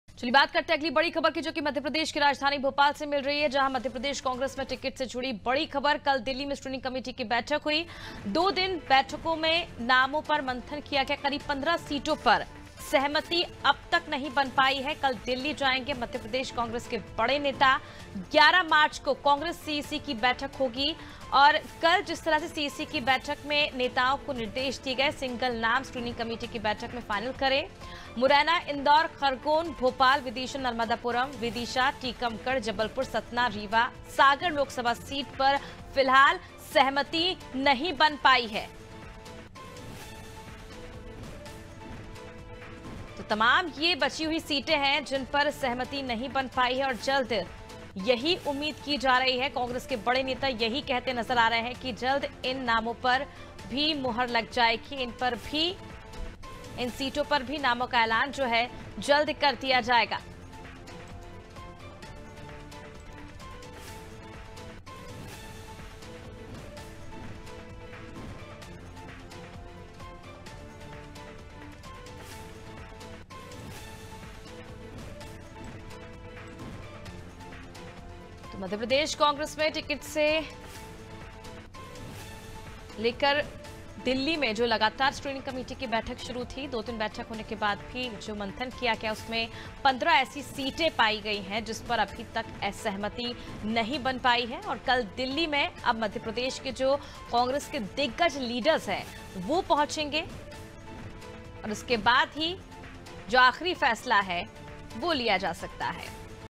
Bhopal : MP कांग्रेस में टिकट से जुड़ी खबर, कल दिल्ली में स्क्रीनिंग कमेटी की बैठक || Anaadi Tv
भोपाल: MP कांग्रेस में टिकट से जुड़ी खबर
कल दिल्ली में स्क्रीनिंग कमेटी की बैठक
2 दिन बैठक में नामों पर होगा मंथन
करीब 15 सीटों पर नहीं बन पाई सहमति
कल दिल्ली जाएंगे MP कांग्रेस के बड़े नेता
11 मार्च को होगी कांग्रेस CEC की बैठक
- कल हुई CEC की बैठक में दिए हैं नेताओं को निर्देश सिंगल नाम स्क्रीनिंग कमेटी के बैठक में फाइनल करें
- मुरैना, इंदौर, खरगोन, भोपाल, विदिशा, नर्मदापुरम, विदिशा, टीकमगढ़, जबलपुर, सतना, रीवा, सागर लोकसभा सीट पर नहीं बन पाई सहमति

AIRTEL - 379